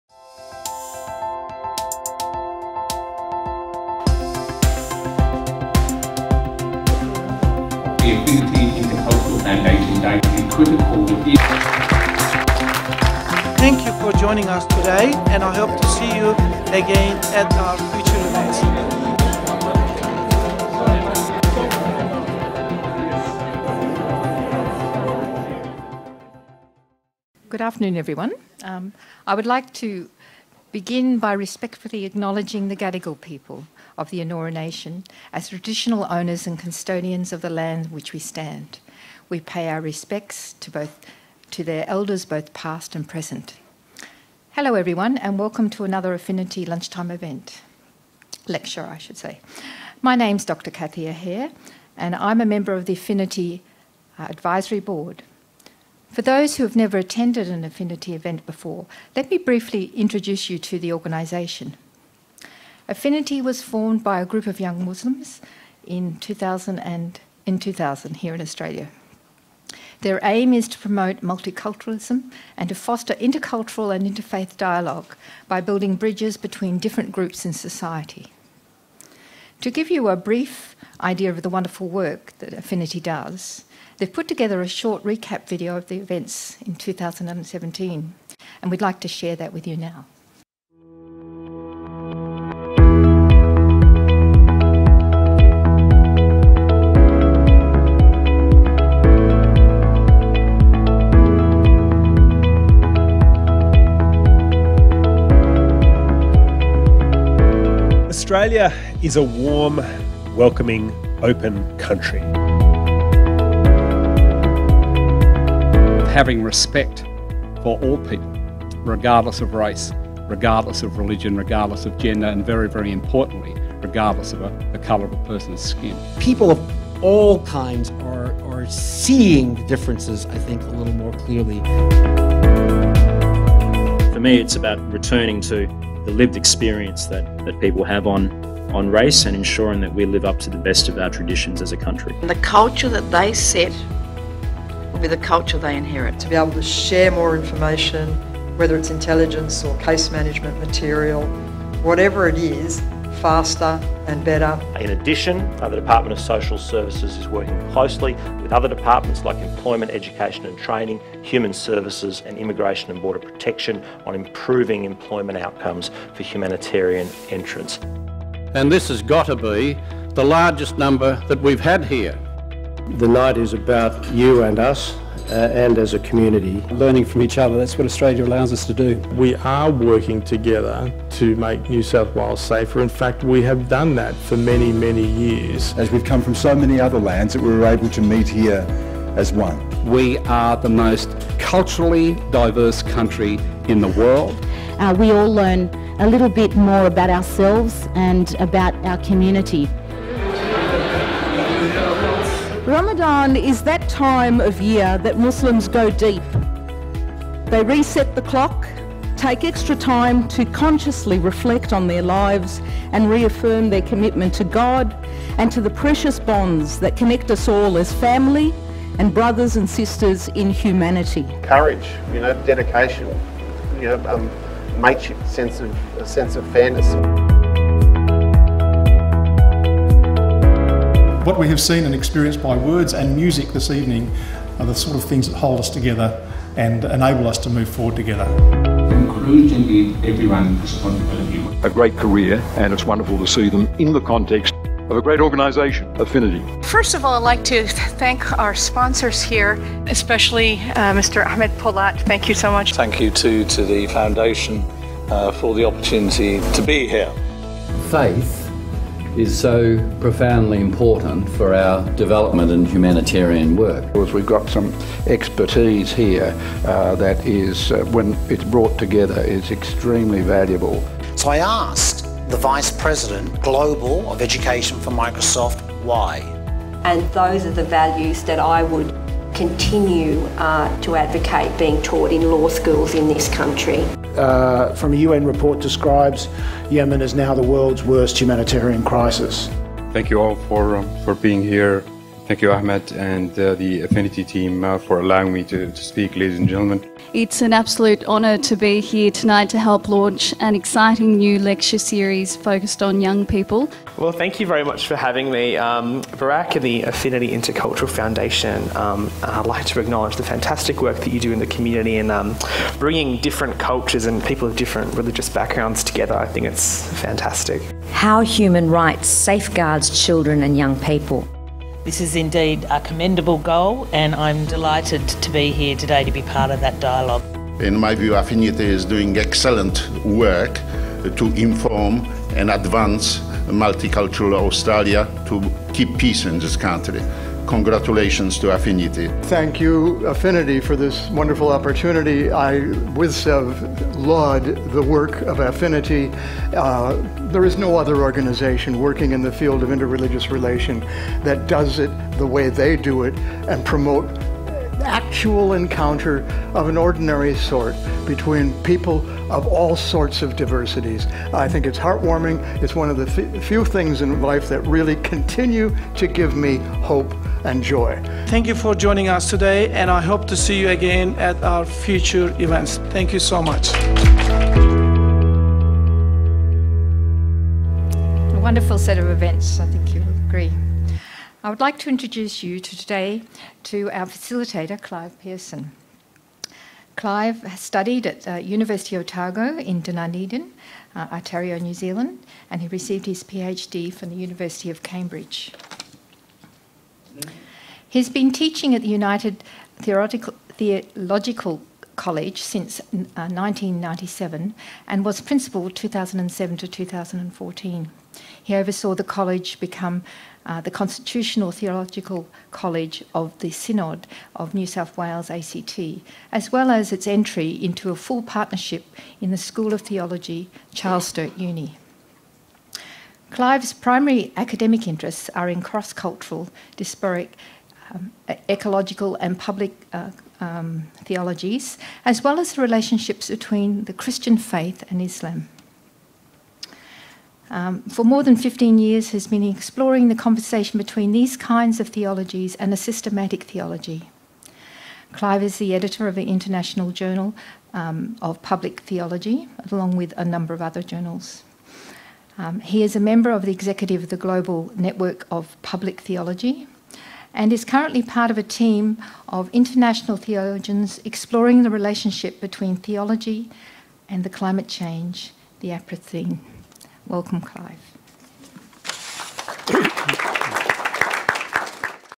Intro Kathy Egea - The Anthropocene: Challenges of the Human Age
‘The Anthropocene: Challenges of the Human Age’ Lecture Series

Emeritus Professor Will Steffen, Fenner School of Environment & Society, Australian National University presents on ‘The Anthropocene: Challenges of the Human Age’ who will be introduced and facilitated by Associate Professor Clive Pearson, Centre for Public and Contextual Theology (PaCT), Charles Sturt University.

Affinity Intercultural Foundation
51-57 Pitt St, Suite 501, Level 5, Sydney, NSW 2000